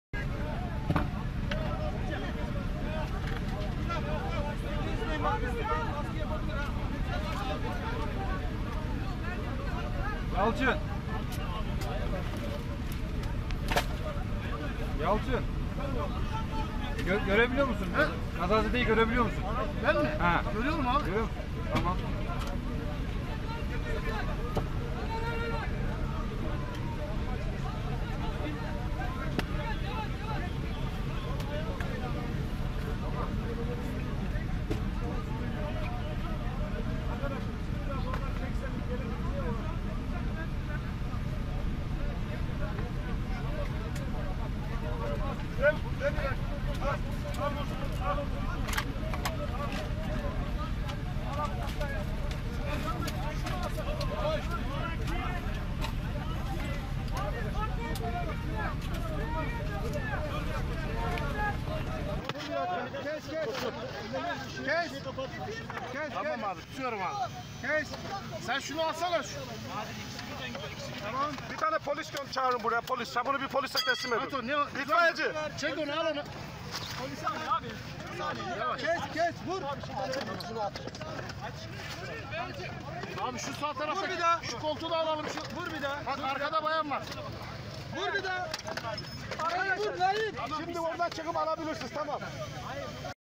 Uçak enkazında yaralıları kurtarma çabası kamerada
Sabiha Gökçen Havalimanı’nda pistten çıkan uçakta itfaiye ve AFAD ekiplerinin yaralıları kurtarma çabası cep telefonu kamerasına yansıdı.